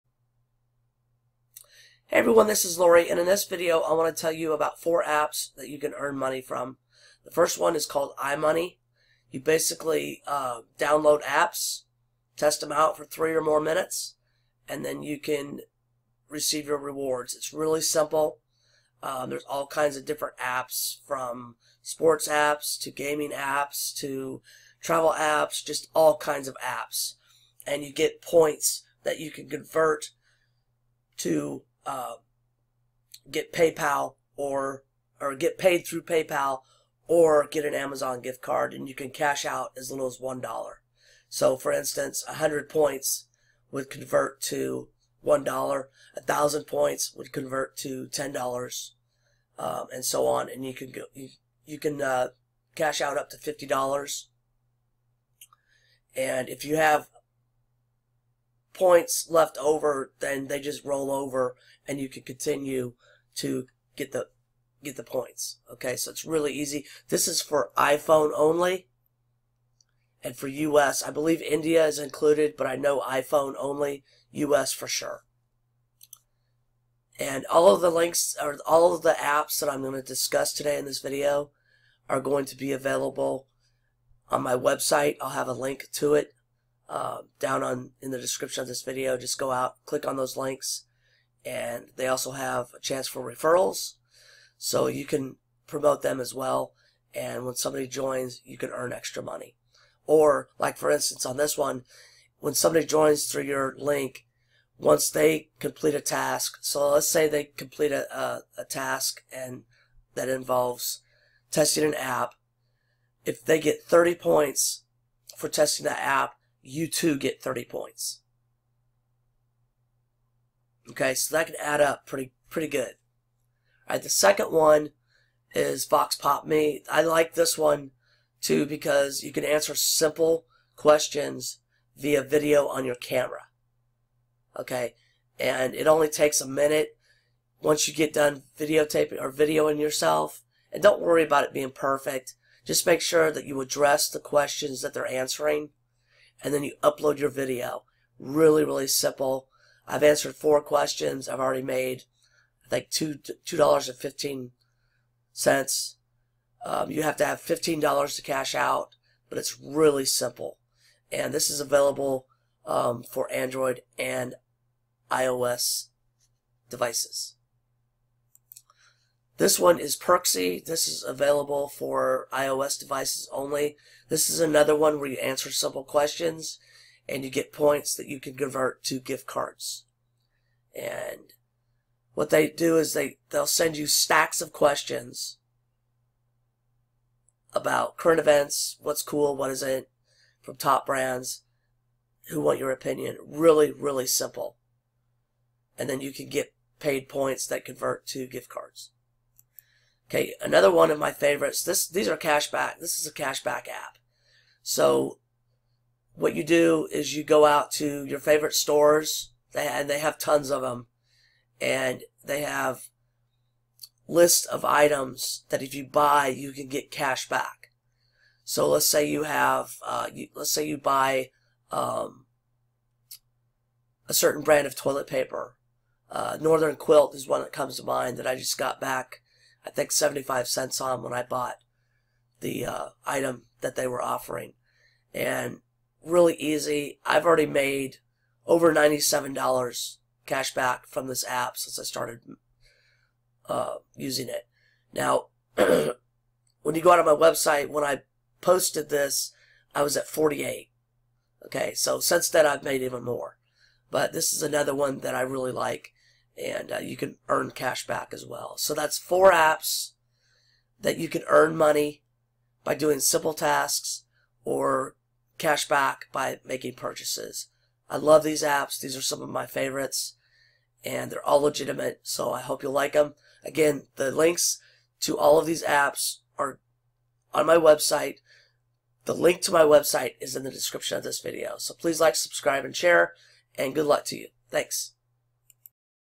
Free Smartphone Apps That Actually Pay | Apps To Make Money
These four apps are legitimate and do pay out to PayPal or gift cards!